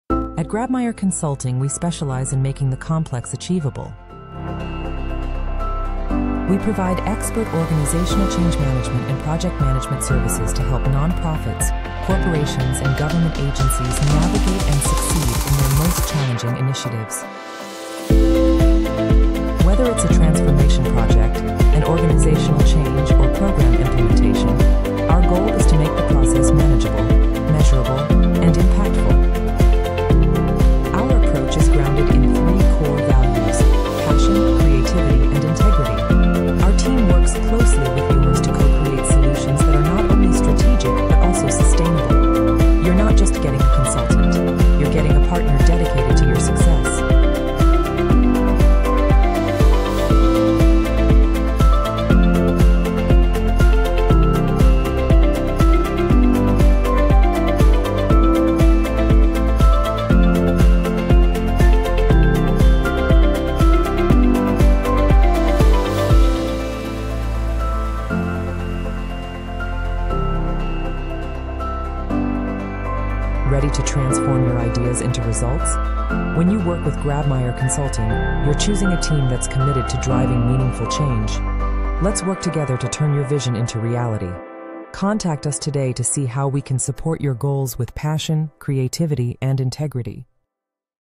Transforming Complexity into Success with Grabemeyer Consulting
At Grabemeyer Consulting, we bring clarity to complex projects and transformations. We’re dedicated to empowering organizations—from government agencies to nonprofits and corporations—to achieve their goals through seamless Organizational Change Management (OCM) and robust project and program management.